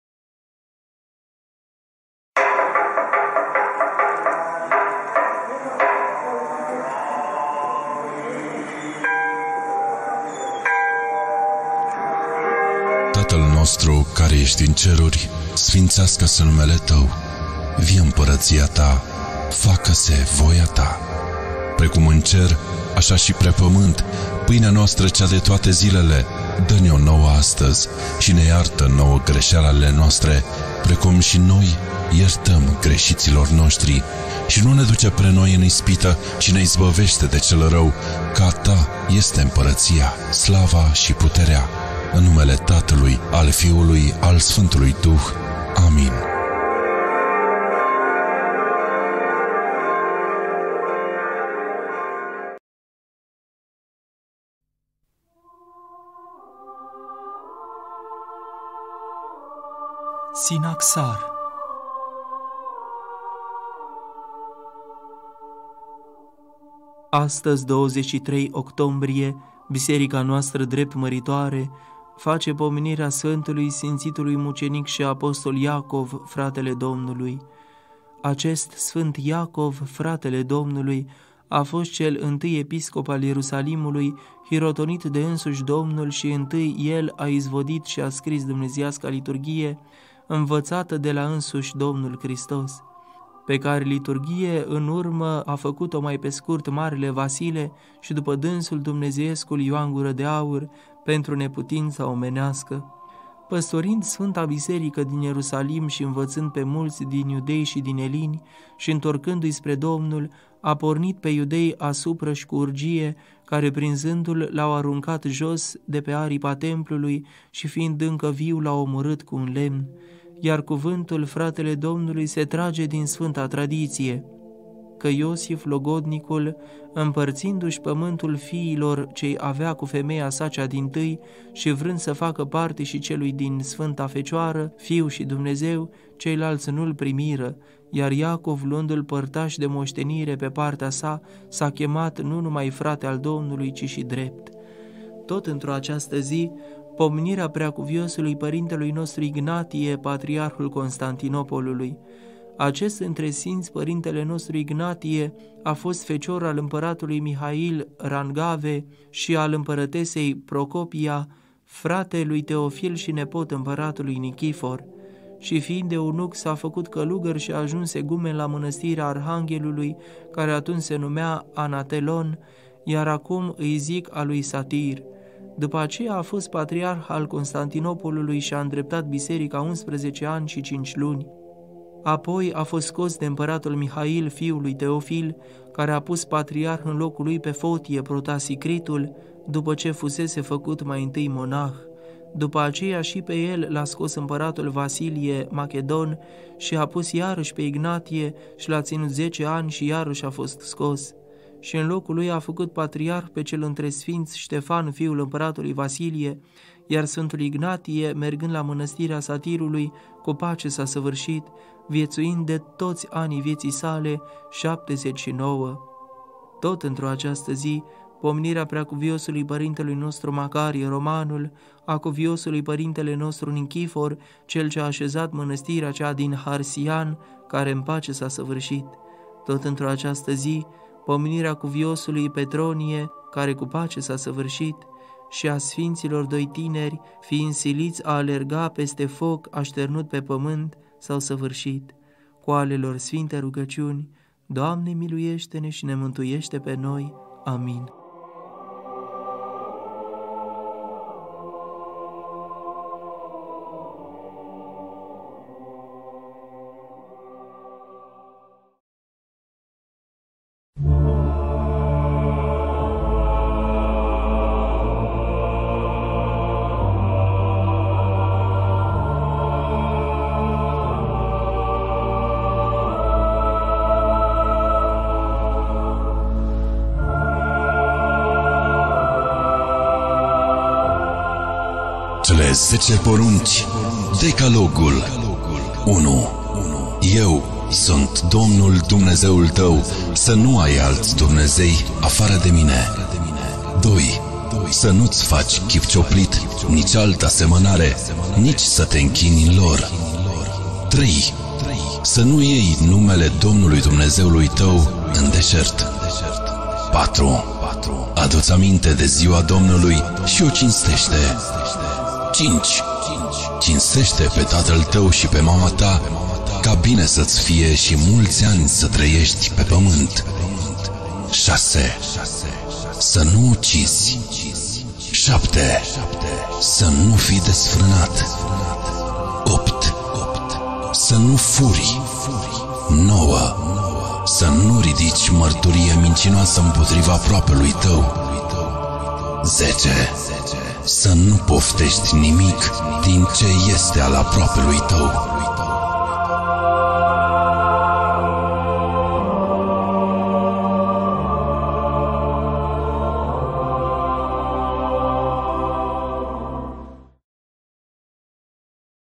Rugăciunea Tatăl Nostru, Sinaxarul zilei de 23.10.2022 și Cele 10 Porunci Dumnezeiești.
Rugăciunea Tatăl Nostru, Sinaxarul zilei de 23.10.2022 și Cele 10 Porunci Dumnezeiești. Cu noi este Dumnezeu, înțelegeți neamuri și vă plecați, căci cu noi este Dumnezeu, În fiecare moment Dumnezeu este cu noi, în afară de momentul când păcătuim, atunci EL se întristează foarte tare și se îndepărtează de la noi.